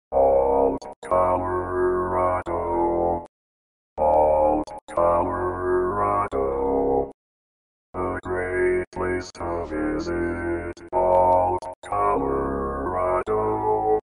How to Say or Pronounce USA Cities — Ault, Colorado
This video shows you how to say or pronounce Ault, Colorado.

A computer said Ault, Colorado. How would you say Ault, Colorado?